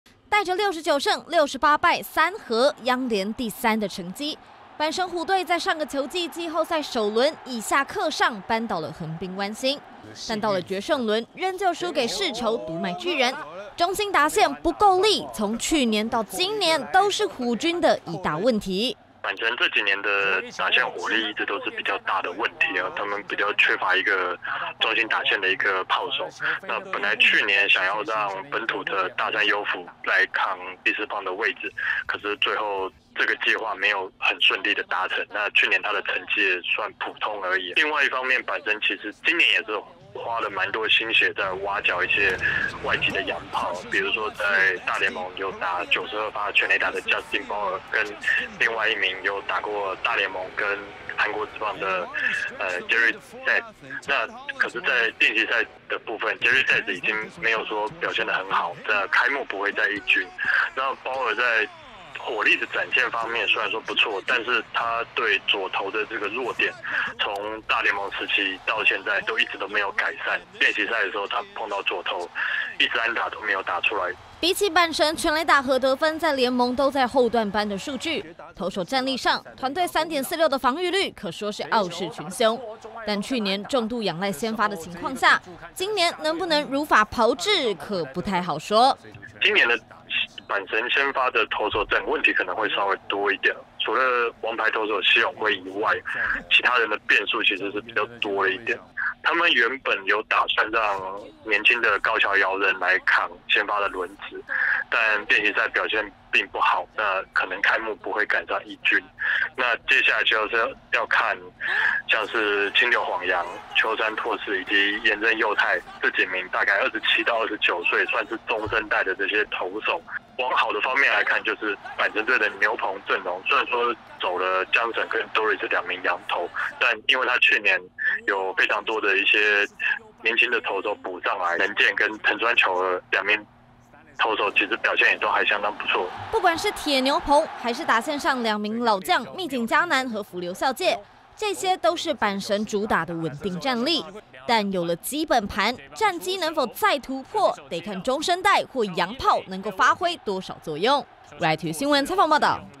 6/19 阪神虎火力求解 苦尋洋砲拒當貧打
說起阪神虎，打線斷層一直是他們頭痛的問題，這幾年不段尋找洋砲求解，今年最受關注的，就是陳偉殷在馬林魚的前隊友Justin Bour的加入。能對虎軍帶來多少影響?請看我們的整理報導